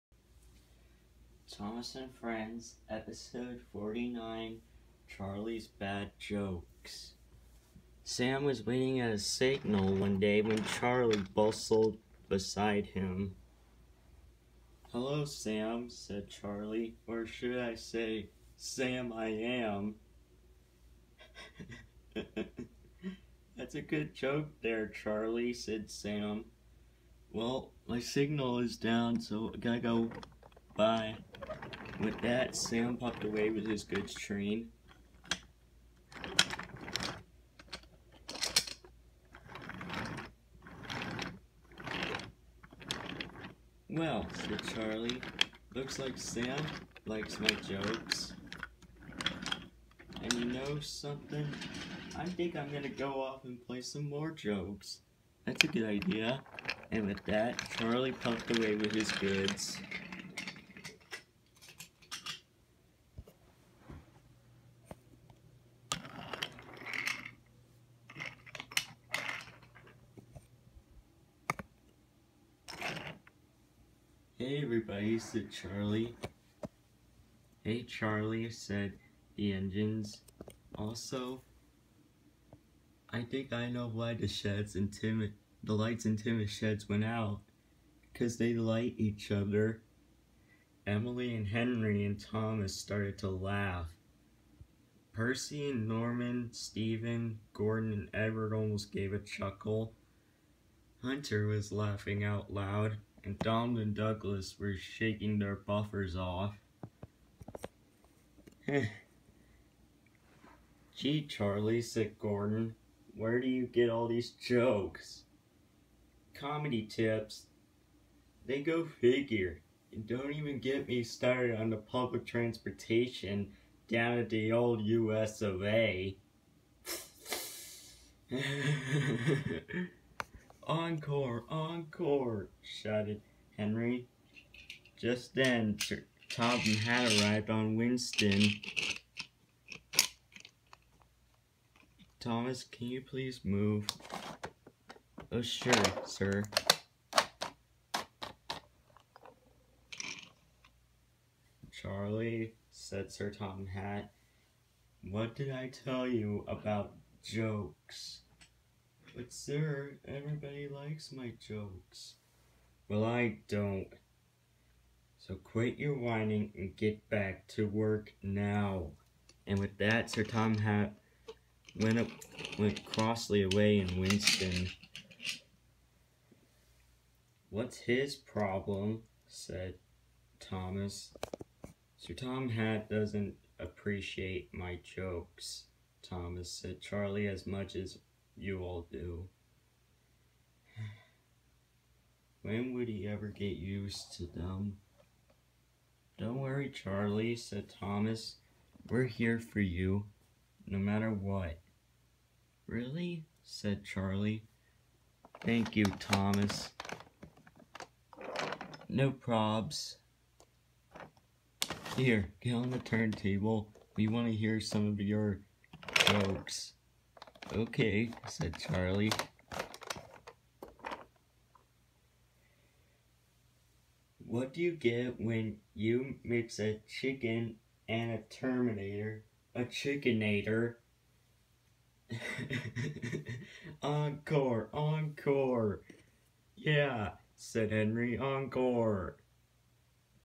49. Charlie's Bad Jokes
Charlie is telling everyone new jokes & they like them.